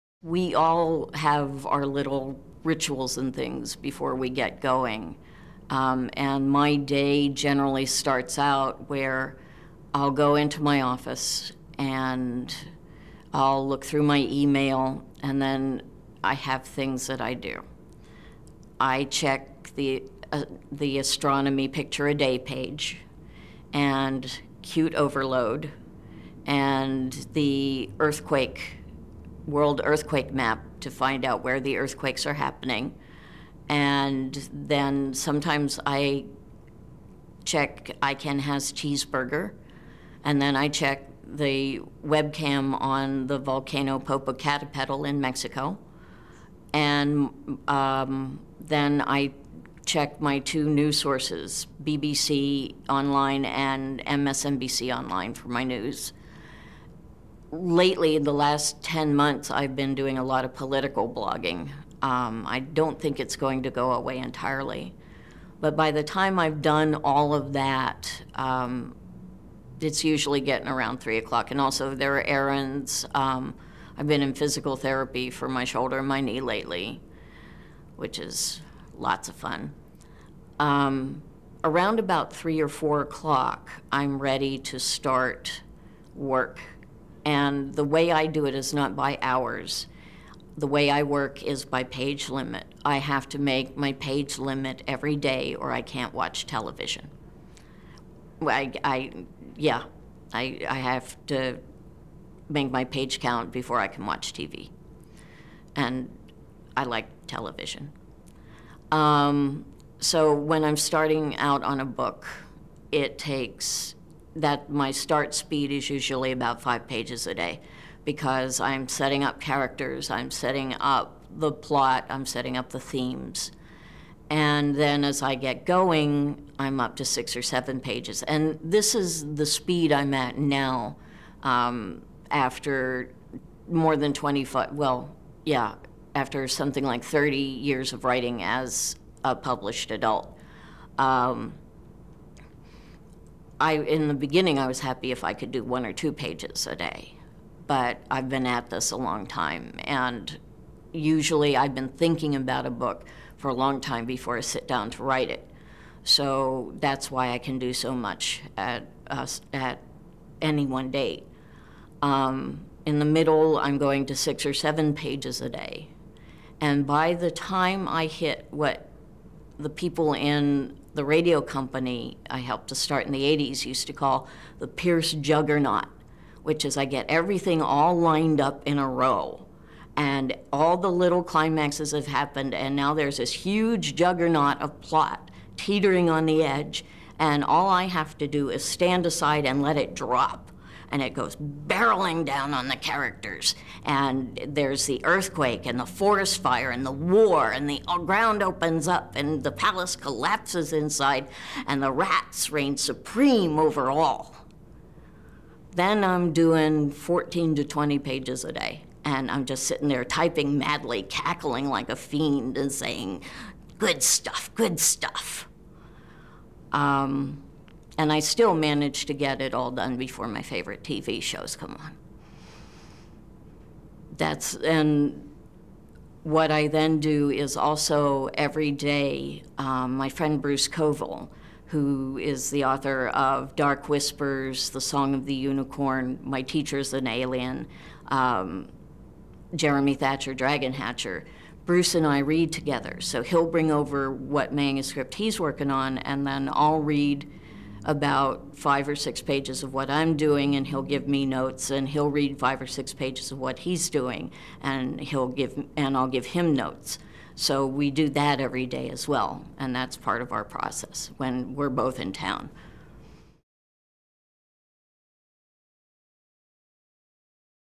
Tamora Pierce has a writing routine
Author Tamora Pierce settles into her writing about 3 or 4pm each day — after checking the days' news and running errands, and she stays at her computer until she reaches her page-count goal for the day.

For more information about teaching reading and writing to adolescents